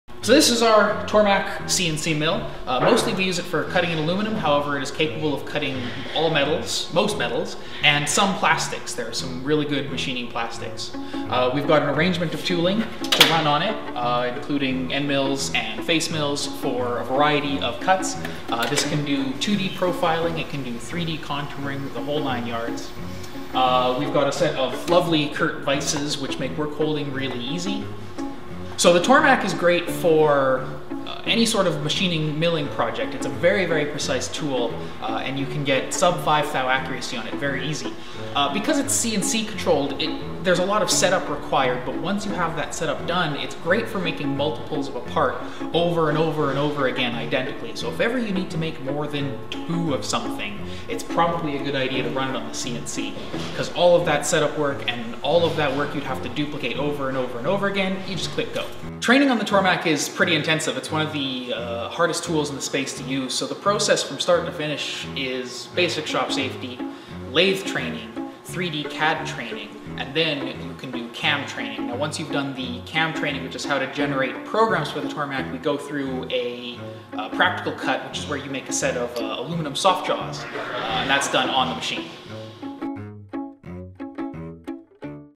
What's at Protospace: The Tormach CNC Mill
Become part of the maker movement. Join your local makerspace. to learn how!
Use of all tools larger than a screwdriver requires the basic shop safety and orientation. Music licensed through the Vimeo Music Store.